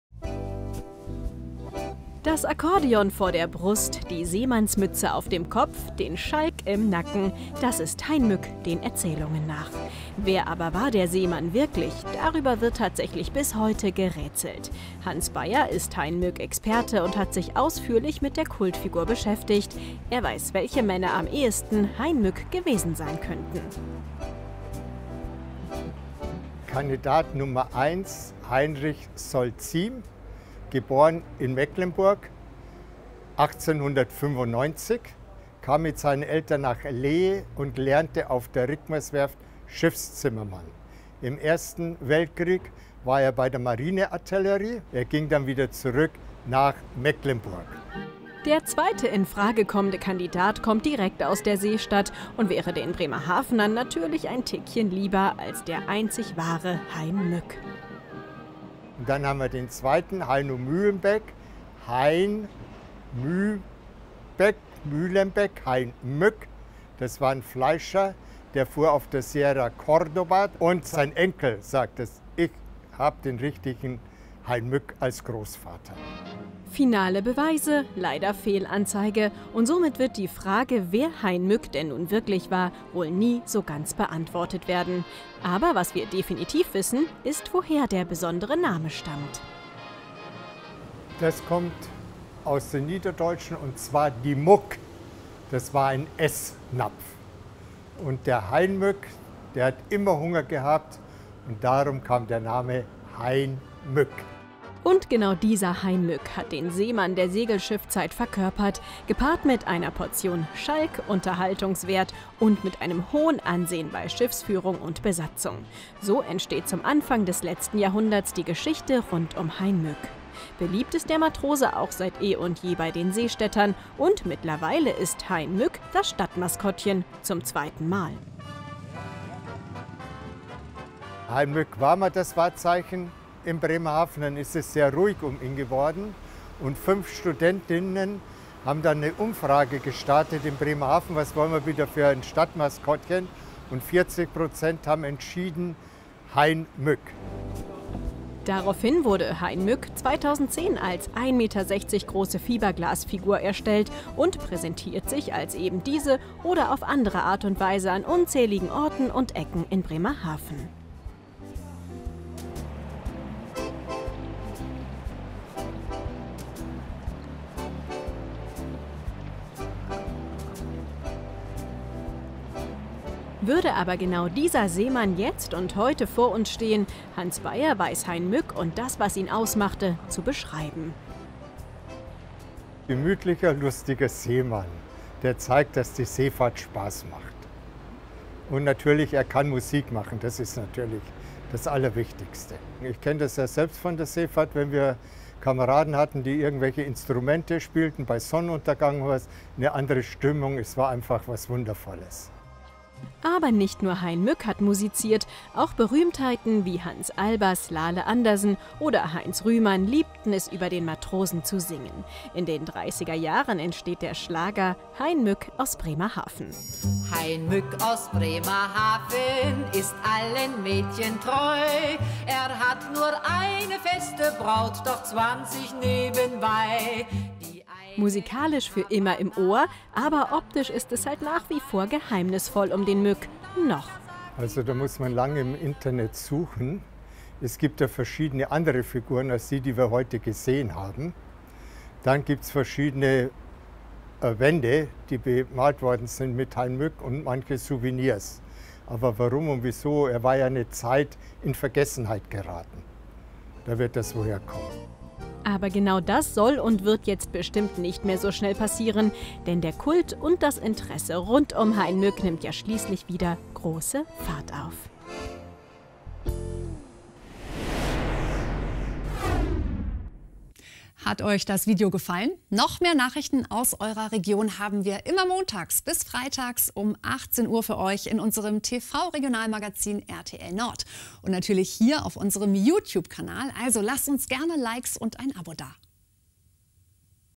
DIE Ikone aus Bremerhaven - Hein Mück
Wer war die Kultfigur und Bremerhavener Stadtikone Heim Mück wirklich? Welcher wahre Mann kann die Vorlage des bekannten Seemanns gewesen sein? Wenn jemand die Antwort kennt, dann Heim Mück Experte Hans Bayer! Wir tauchen ein in das Leben des Seemanns aus der Segelschiffzeit, das heutige Maskottchen der Stadt Bremerhaven.